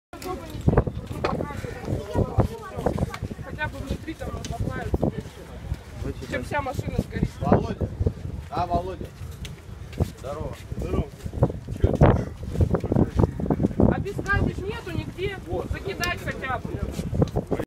На улице Татьяны Снежиной загорелся автомобиль
На улице Татьяны Снежиной загорелся автомобиль. На место прибыли пожарные. Очевидец сообщает, что благодаря свидетелям происшествия удалось спасти автомобиль "Субару", стоявший ряд с загоревшимся авто — люди оттащили его в сторону.